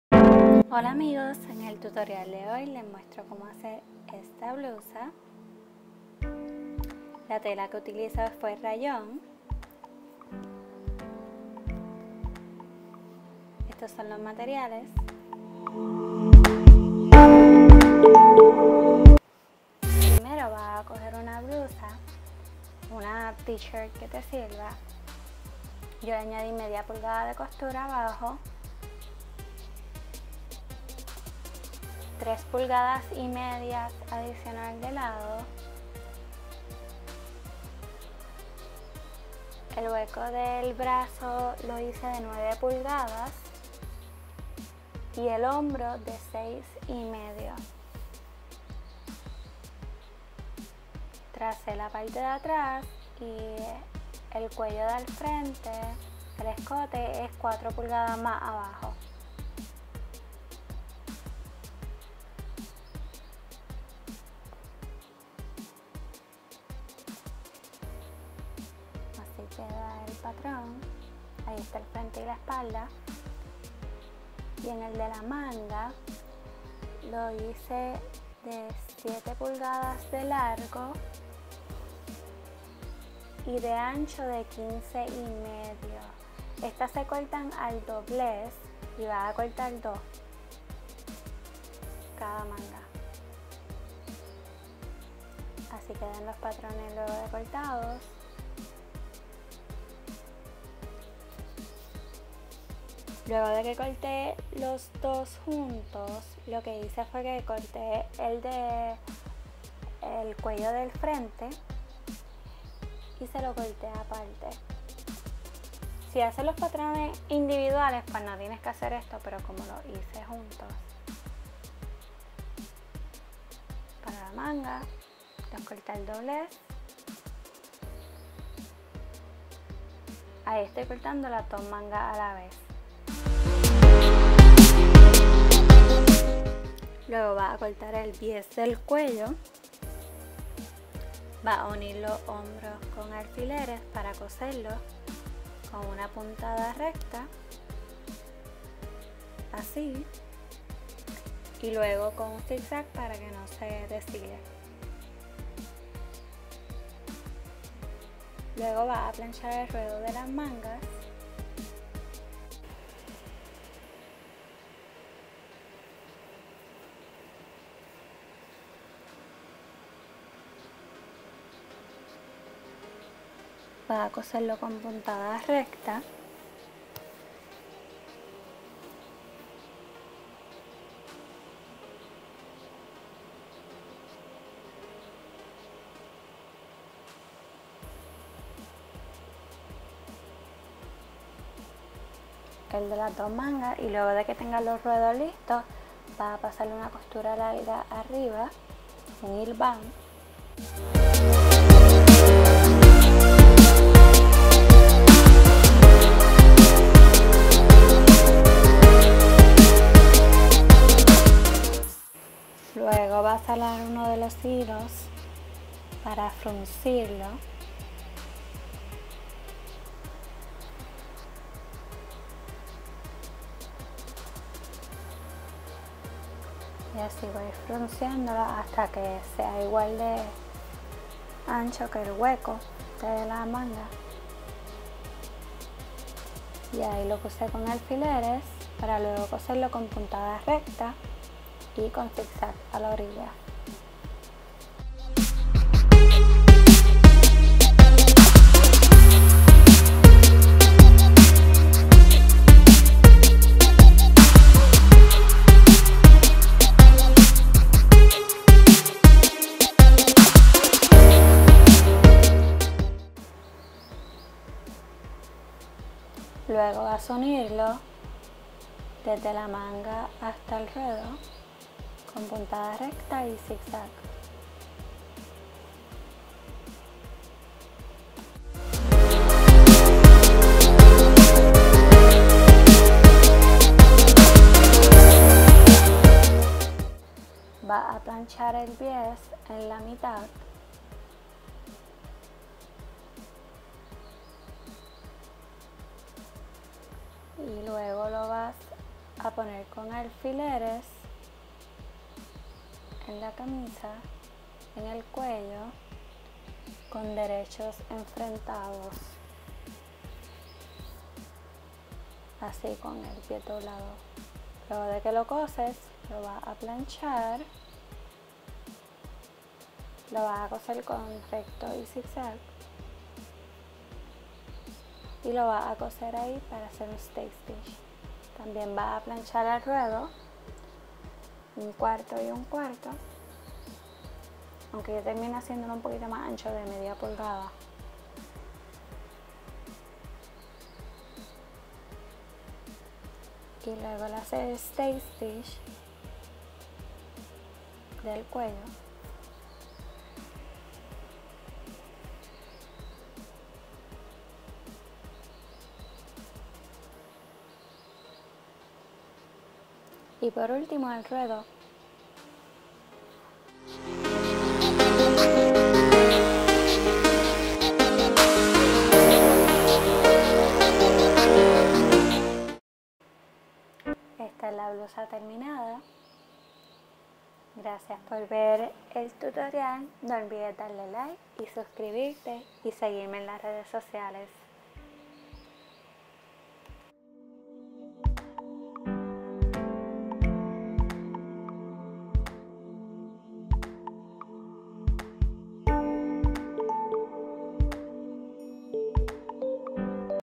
DIY blusa con mangas de volantes / DIY flared sleeve top
DIY blusa con mangas de volantes. Paso a Paso de como hacer la blusa.Ropa de mujer, diy ropa, clases de costura, patrones, diy, blusa... COMENTA PARA MÁS INFORMACIÓN!

Blusa de volantes